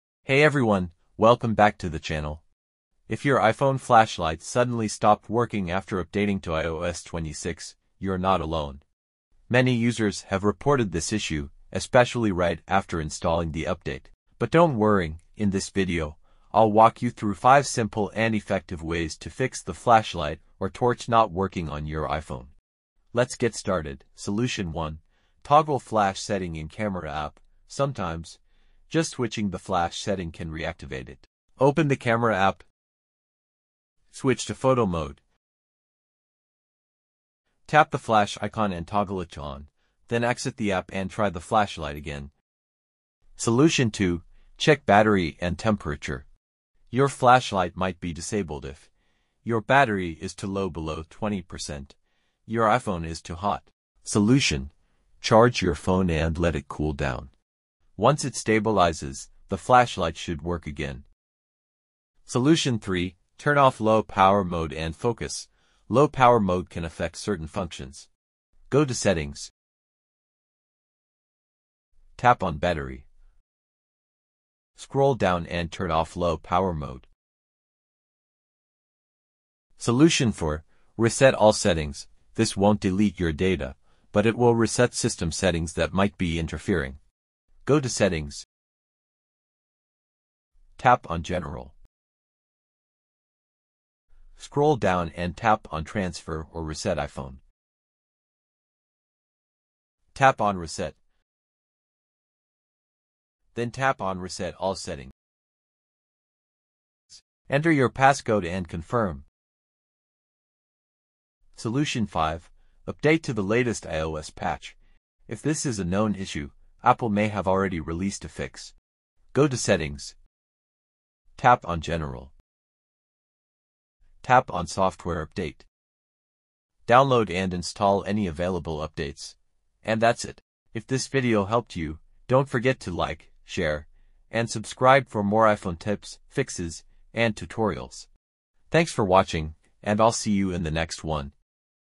How to Fix Flashlight On iPhone | iOS 26 Flashlight Issue
Is your iPhone flashlight not working or stuck? Don’t worry! In this quick video, I’ll show you 5 simple methods to fix your iPhone flashlight issue, whether it’s a software glitch, battery setting, or iOS bug.

📱 Covered Methods in This Video:
1️⃣ Toggle Flash Setting in Camera App
2️⃣ Check Battery & iPhone Temperature
3️⃣ Turn Off Low Power Mode & Focus
4️⃣ Reset All Settings
5️⃣ Update to the Latest iOS Patch

💡 These solutions work for all iPhones including iPhone 11, 12, 13, 14, and newer.
✅ Try these steps before visiting a service center!